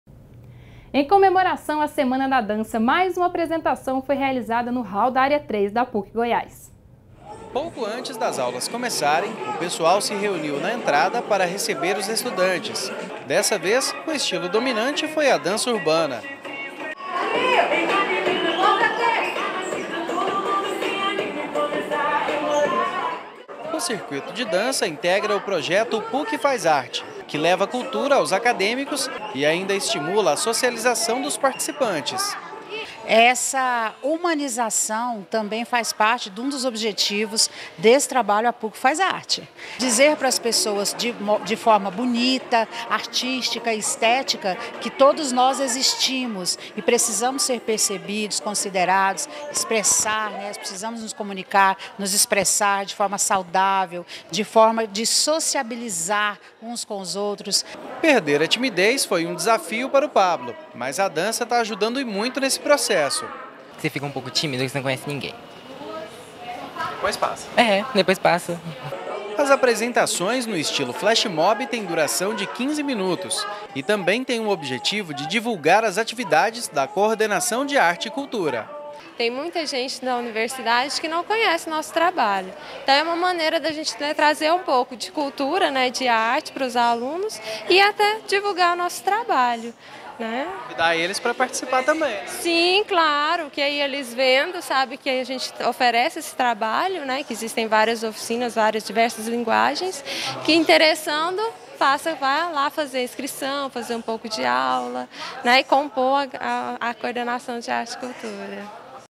Veja mais uma apresentação de dança na área 3 da PUC Goiás!
O momento de diversão foi realizado em comemoração a semana da dança. O projeto PUC Faz Arte promove apresentações culturais que promovem a interação entre as pessoas. Dessa vez o estilo de dança foi a urbana.
INFORMAÇÕES:
Assista: